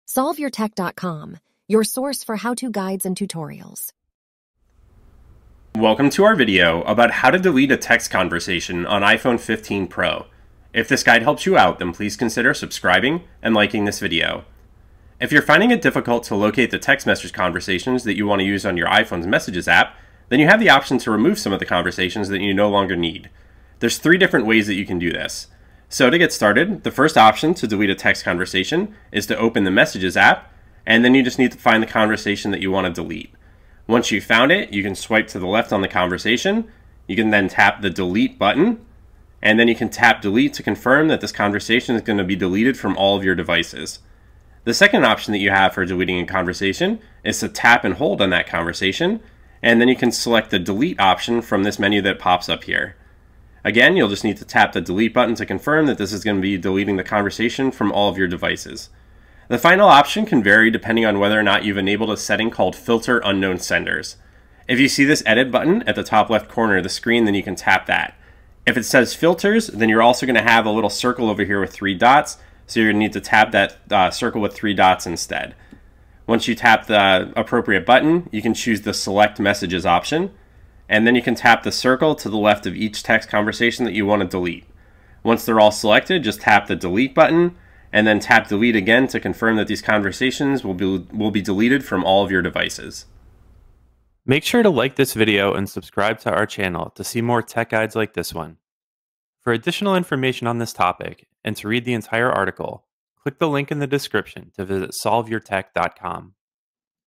How to Delete Text Conversation on iPhone 15 Pro
This video will show you how to delete a text conversation on iPhone 15 Pro. Once you complete this tutorial, you will know how to remove a single conversation from your Messages app.

You can delete an iPhone 15 Pro text conversation with the following steps:

1. Open the Messages app.
2. Tap Edit at the top-left.
3. Choose Select Messages.
4. Tap the circle to the left of the conversation to delete.
5. Tap Delete.

Alternatively, you can swipe left on the conversation, then tap the Delete button.

00:00 Introduction
00:14 How to Delete Text Conversation on iPhone 15 Pro
01:33 Outro

Related Questions and Issues This Video Can Help With:

- How to Delete Text Conversation on iPhone 15 Mini
- How to Delete Text Conversation on iPhone 15 Plus
- How to Delete Text Conversation on iPhone 15 Pro Max